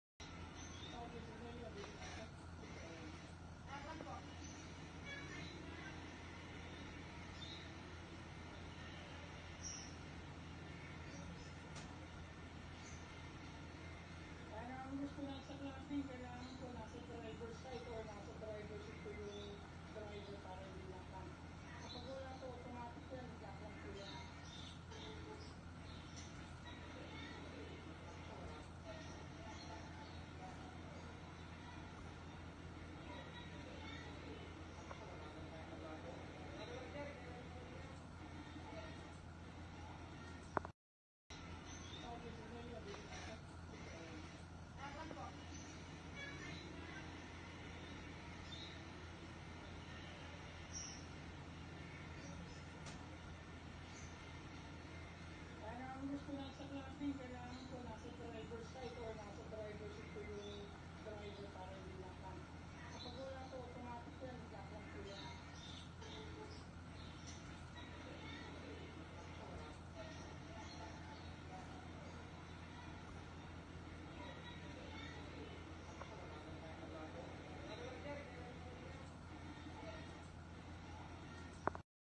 DIBDIB NG 9 NA BUWANG BUNTIS, DINAKMA NG ISANG RIDER SA TAGUIG
Paliwanag ng biktima na nagtatanong ang nasabing driver ng direksyon matapos nito ay bigla na lamang dinakma ng suspek ang dibdib ng 9 na buwang buntis na babae, nakuhanan pa ito ng CCTV, nananawagan naman ang biktima sa makakapagturo sa suspek